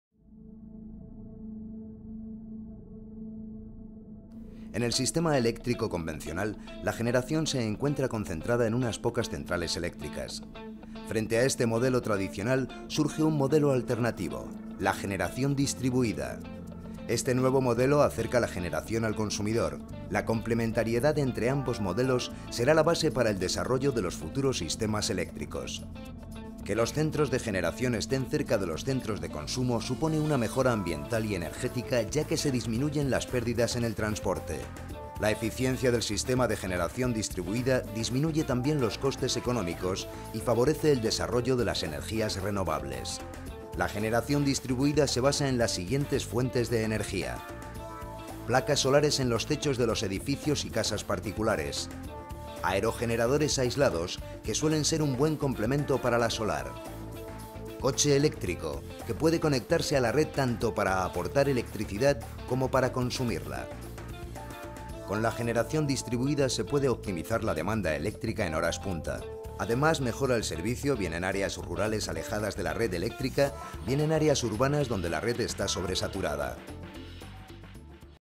Cómo funciona la generación distribuida
La generación distribuida o descentralizada es una parte fundamental en una Smart City, y consiste en la generación de energía eléctrica mediante muchas pequeñas fuentes de generación, instaladas cerca del consumo. La generación distribuida es una cooperación entre la micro-generación  y la generación de las centrales convencionales. Esta distribución hace que la generación sea más equilibrada, y que la Smart City no dependa tanto de las grandes centrales. Además, la micro-generación implica el uso de las energías renovables, lo que reducirá las emisiones de CO2 (fuente: Endesa).